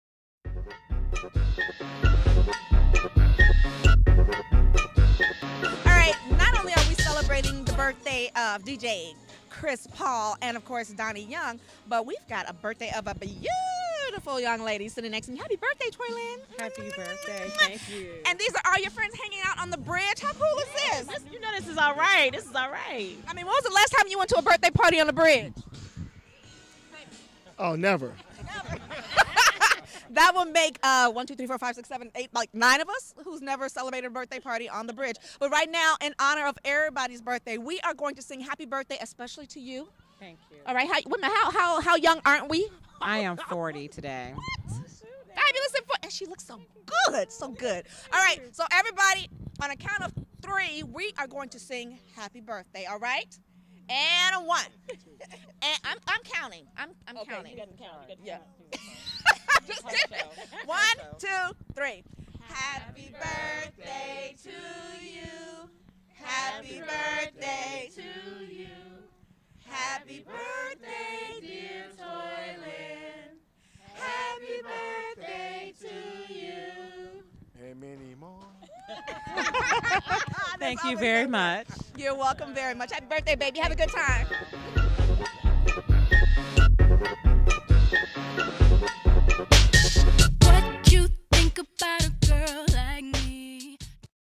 HappiBdai 'On the Couch' Toilynn
her friends gathered on the Purple People Bridge to celebrate her FabForthieth!!!